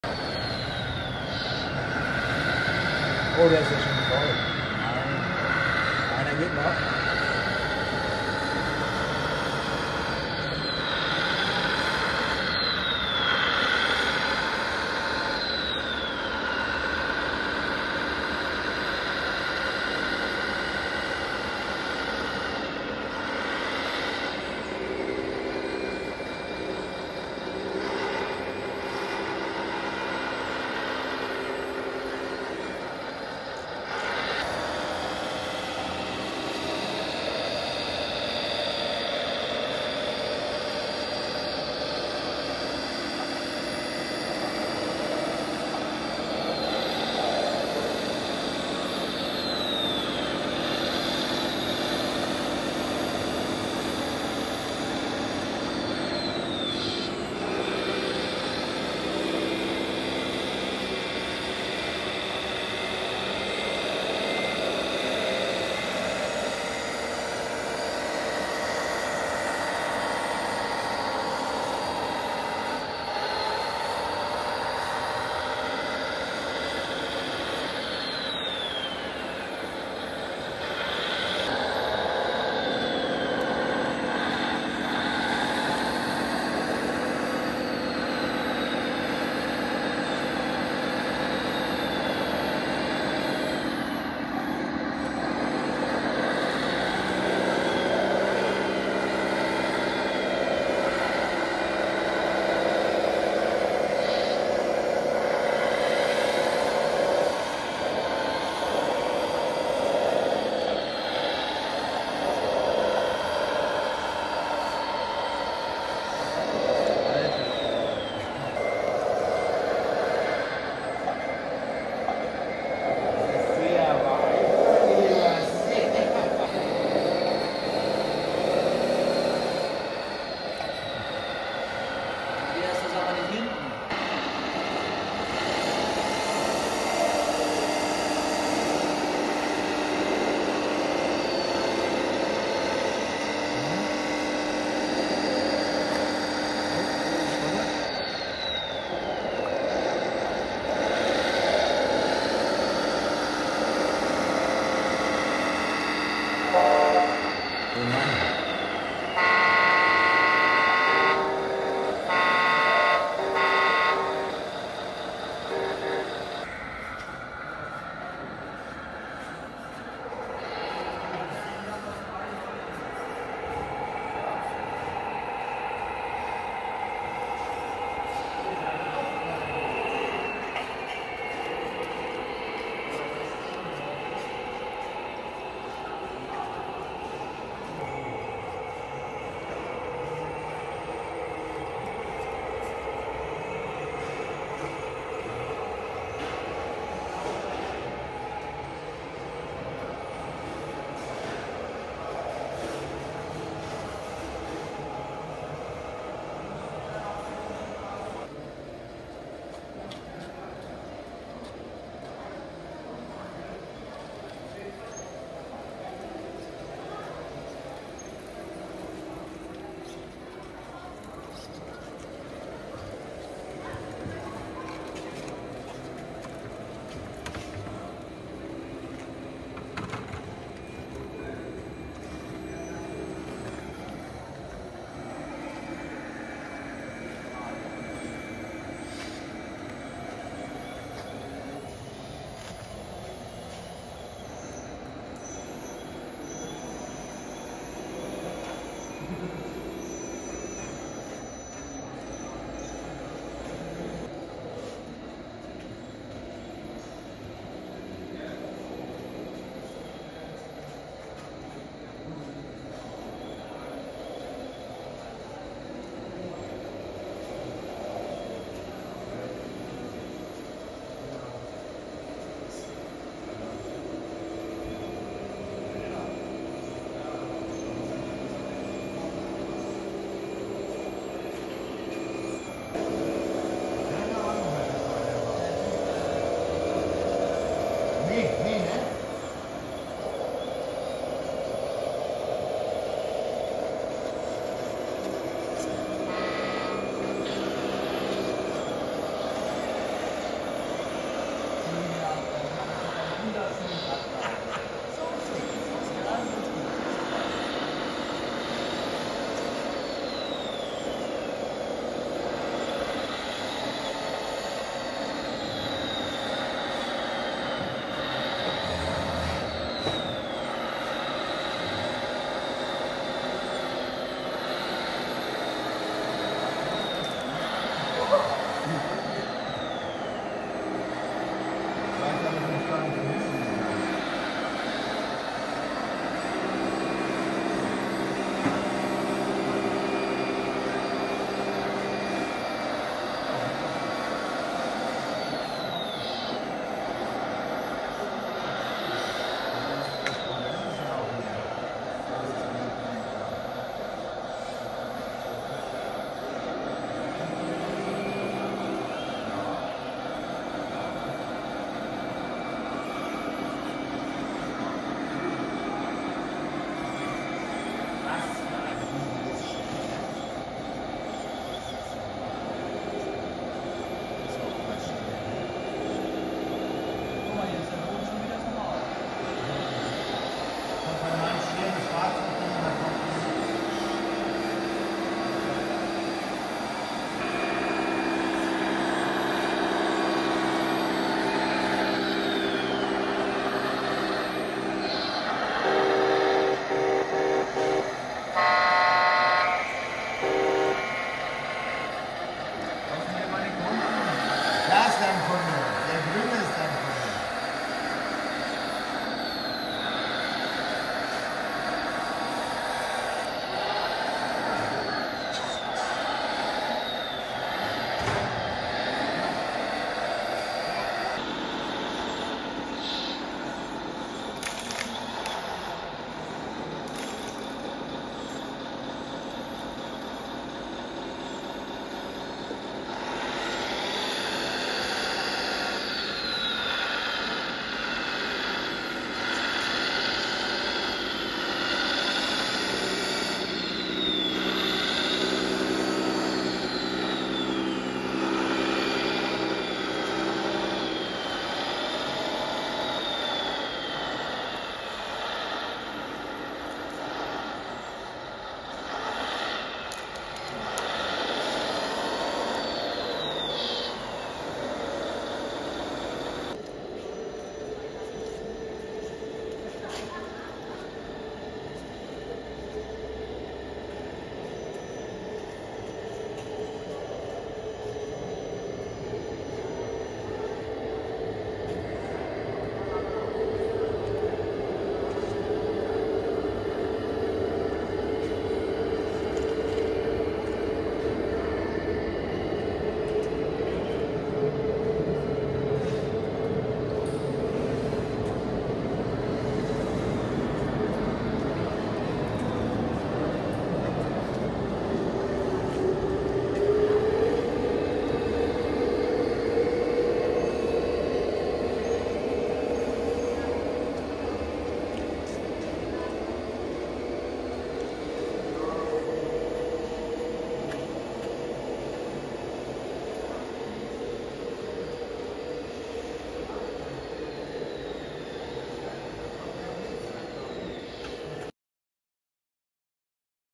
Building Site Action Wheel Loader Trucks Tractor im RC Glashaus 4K UHD
RC Baustelle im RC Glashaus, Radlader und Trucks bei der Arbeit.